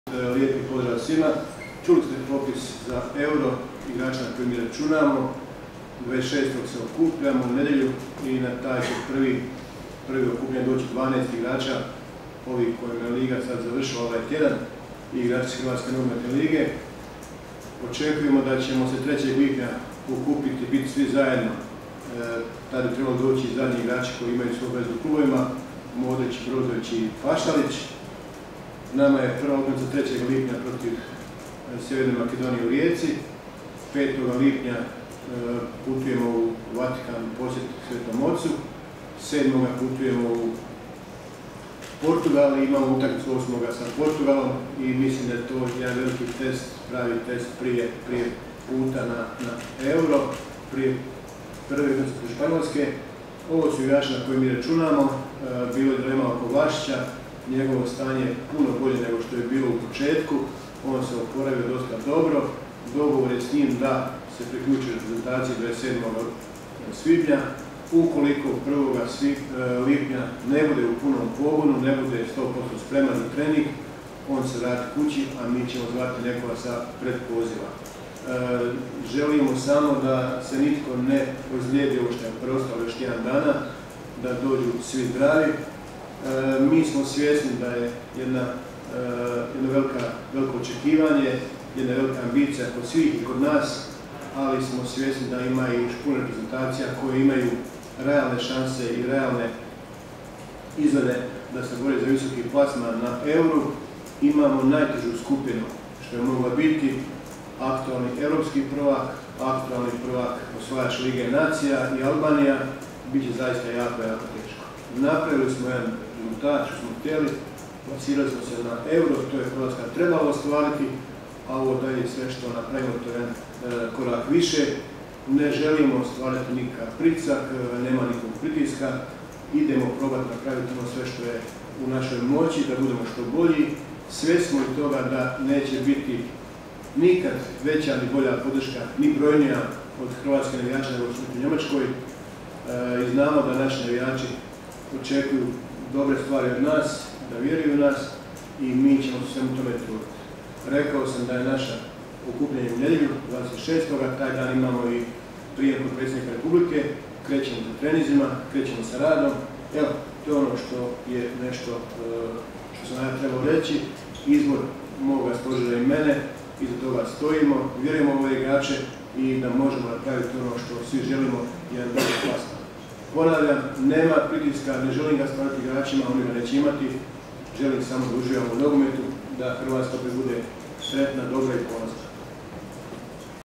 Dalić uoči EURA: "Imamo najtežu moguću skupinu, bit će zaista jako, jako teško"
Predstavljena nova Žujina nogometna kampanja i preliminarni popis igrača za predstojeće Europsko prvenstvo u Njemačkoj.